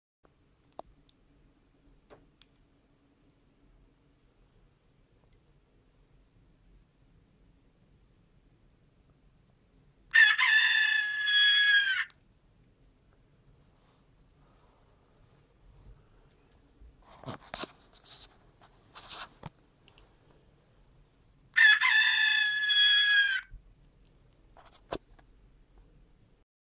Waking Steve up with the chicken!
I bought a small chicken key ring in Valencia and every away game I wake Steve up with the 'cock-a-doodle-doo'.